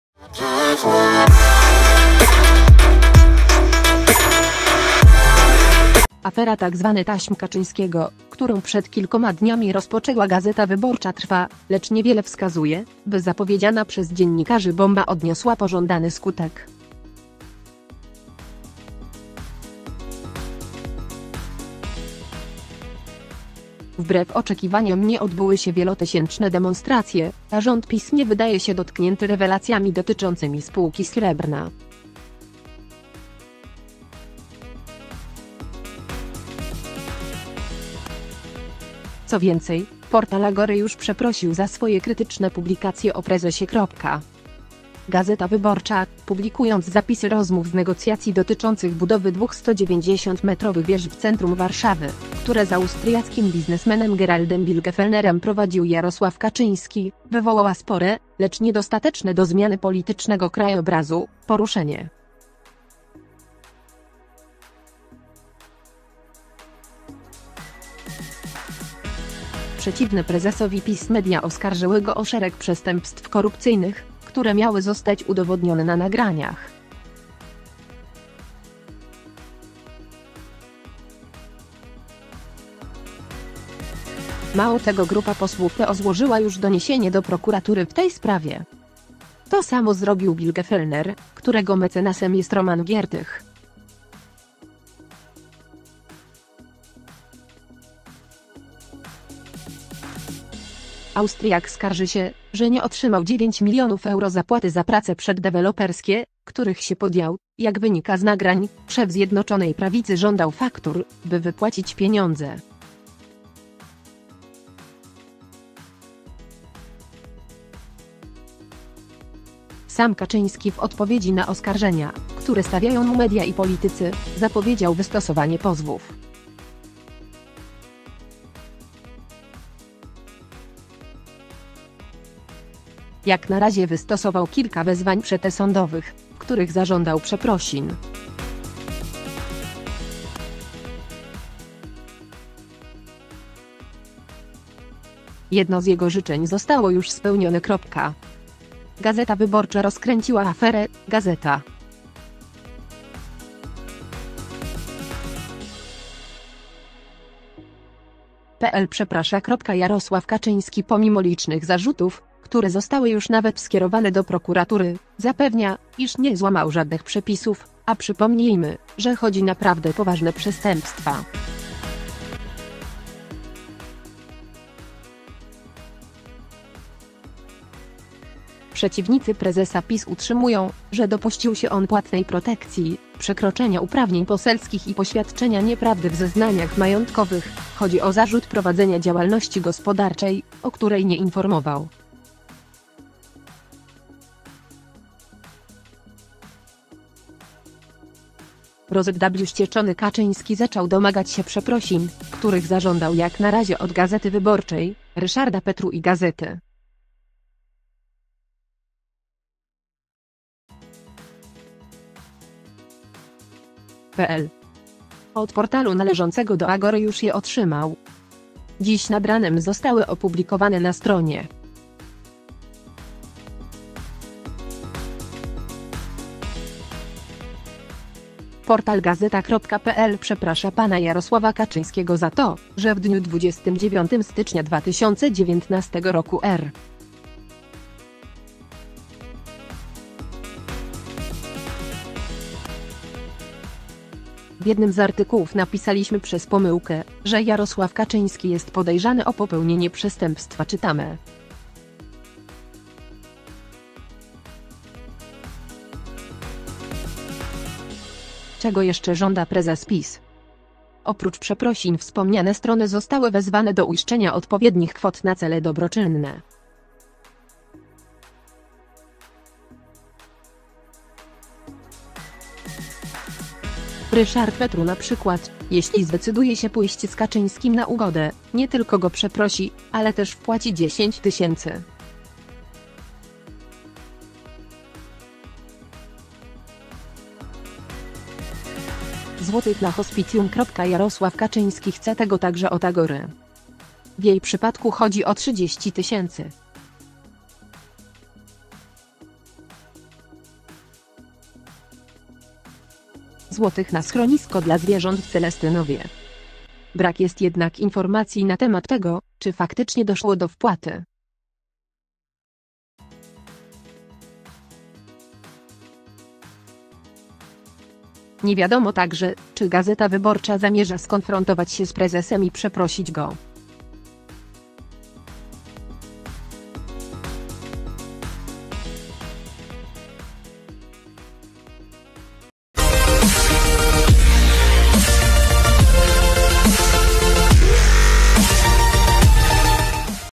Kaczyński zmusił portal Agory do przeprosin. Szef PiS domaga się też wielkiego zadośćuczynienia | Ak
Afera tzw. taśm Kaczyńskiego, którą przed kilkoma dniami rozpoczęła „Gazeta Wyborcza” trwa, lecz niewiele wskazuje, by zapowiedziana przez dziennikarzy „bomba” odniosła pożądany skutek. Wbrew oczekiwaniom nie odbyły się wielotysięczne demonstracje, a rząd PiS nie wydaje się dotknięty rewelacjami dotyczącymi Spółki Srebrna. Co więcej, portal Agory już przeprosił za swoje krytyczne publikacje o prezesie.

„Gazeta Wyborcza”, publikując zapisy rozmów z negocjacji dotyczących budowy dwóch 190- metrowych wież w centrum Warszawy, które z austriackim biznesmenem Geraldem Bilgefellnerem prowadził Jarosław Kaczyński, wywołała spore, lecz niedostateczne do zmiany politycznego krajobrazu, poruszenie. Przeciwne prezesowi PiS media oskarżyły go o szereg przestępstw korupcyjnych, które miały zostać udowodnione na nagraniach.

Mało tego grupa posłów PO złożyła już doniesienie do prokuratury w tej sprawie. To samo zrobił Bilgefellner, którego mecenasem jest Roman Giertych. Austriak skarży się, że nie otrzymał 9 milionów euro zapłaty za prace przeddeweloperskie, których się podjął (jak wynika z nagrań, szef Zjednoczonej Prawicy żądał faktur, by wypłacić pieniądze).
Sam Kaczyński w odpowiedzi na oskarżenia, które stawiają mu media i politycy, zapowiedział wystosowanie pozwów. Jak na razie wystosował kilka wezwań przedsądowych, w których zażądał przeprosin. Jedno z jego życzeń zostało już spełnione.

„Gazeta Wyborcza” rozkręciła aferę, Gazeta.pl przeprasza
Jarosław Kaczyński pomimo licznych zarzutów, które zostały już nawet skierowane do prokuratury, zapewnia, iż nie złamał żadnych przepisów, a przypomnijmy, że chodzi naprawdę poważne przestępstwa. Przeciwnicy prezesa PiS utrzymują, że dopuścił się on płatnej protekcji, przekroczenia uprawnień poselskich i poświadczenia nieprawdy w zeznaniach majątkowych (chodzi o zarzut prowadzenia działalności gospodarczej, o której nie informował).

Rozwścieczony Kaczyński zaczął domagać się przeprosin, których zażądał jak na razie od „Gazety Wyborczej”, Ryszarda Petru i Gazety.pl. Od portalu należącego do Agory już je otrzymał. Dziś nad ranem zostały opublikowane na stronie.

– Portal Gazeta.pl przeprasza pana Jarosława Kaczyńskiego za to, że w dniu 29 stycznia 2019 r. w jednym z artykułów napisaliśmy przez pomyłkę, że Jarosław Kaczyński jest podejrzany o popełnienie przestępstwa – czytamy.

Czego jeszcze żąda prezes PiS?
Oprócz przeprosin wspomniane strony zostały wezwane do uiszczenia odpowiednich kwot na cele dobroczynne. Ryszard Petru np., jeśli zdecyduje się pójść z Kaczyńskim na ugodę, nie tylko go przeprosi, ale też wpłaci 10 tys. złotych na hospicjum.

Jarosław Kaczyński chce tego także od Agory. W jej przypadku chodzi o 30 tys. złotych na schronisko dla zwierząt w Celestynowie. Brak jest jednak informacji na temat tego, czy faktycznie doszło do wpłaty. Nie wiadomo także, czy „Gazeta Wyborcza” zamierza skonfrontować się z prezesem i przeprosić go.